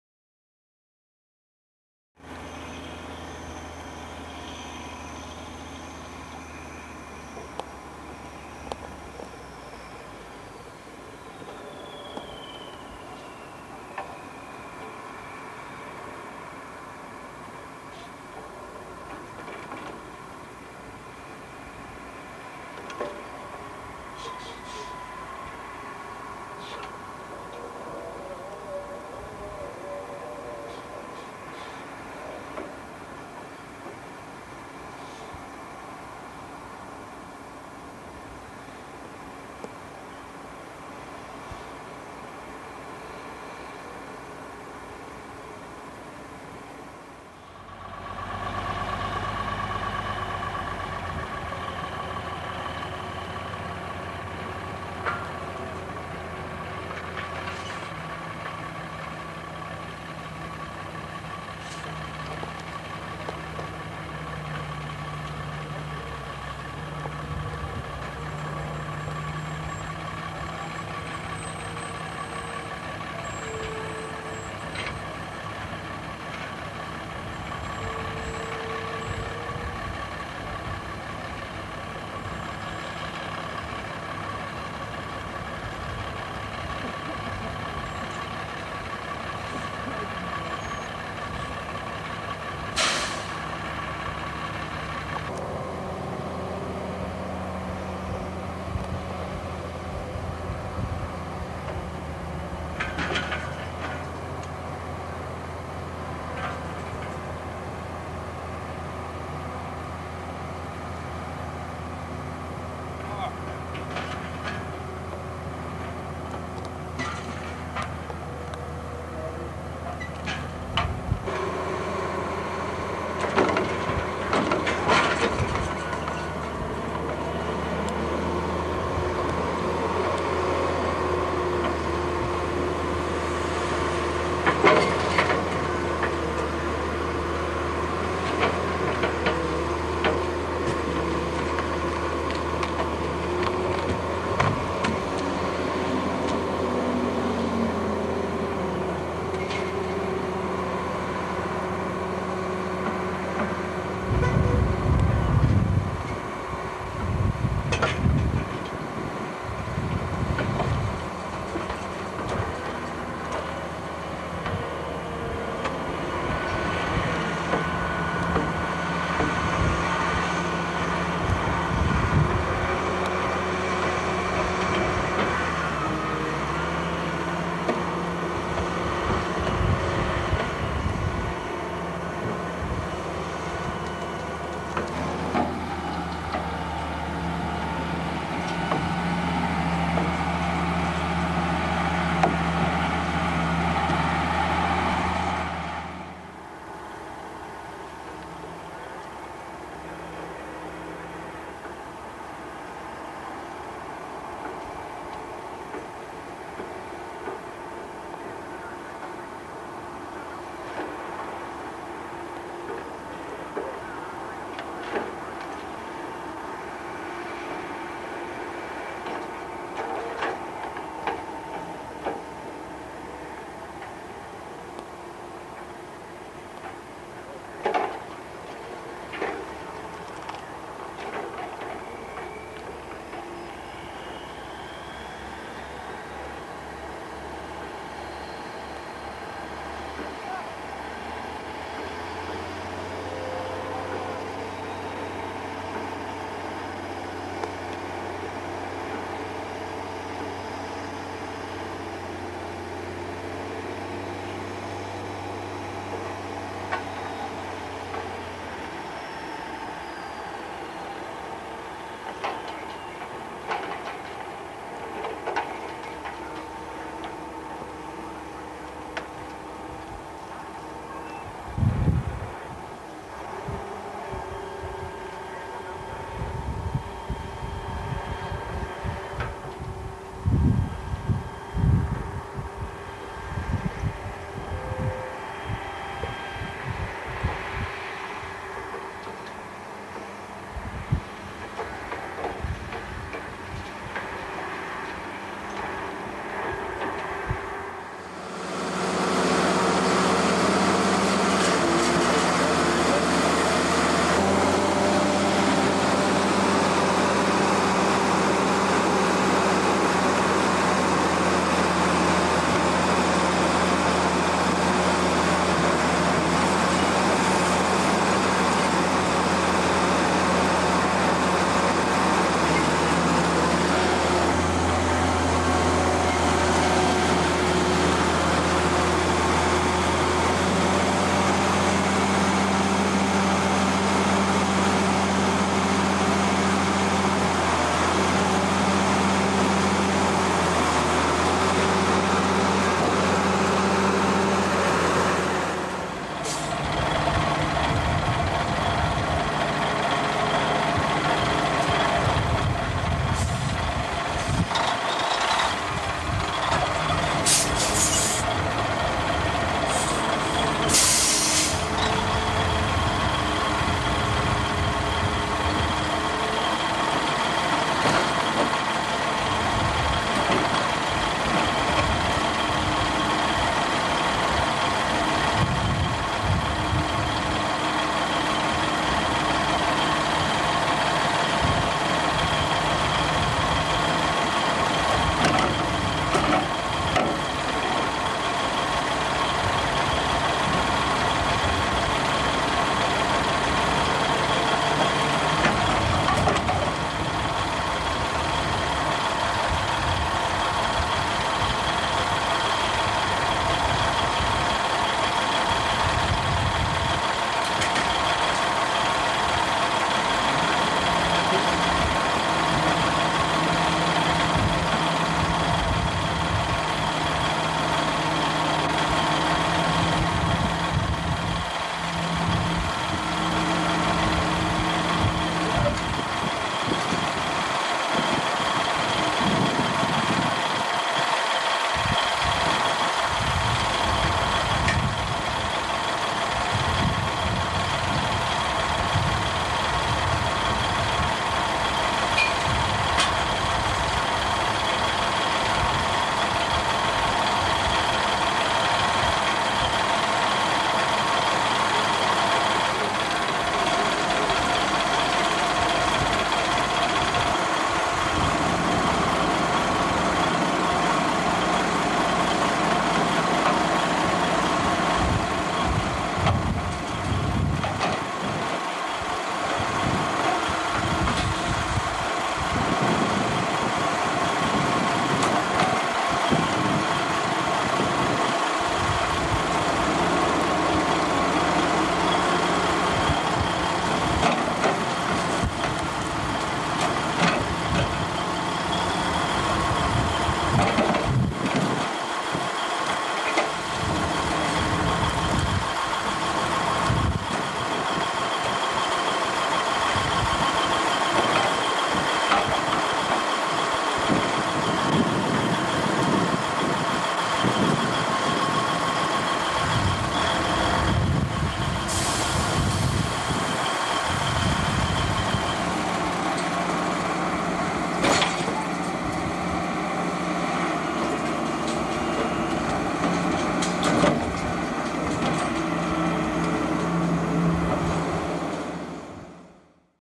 North Ryan - Loading Trucks
North Ryan - Valleau Logging 1990's
This video was filmed and donated by Rick Valleau.
This clip shows the set up and loading of logging trucks.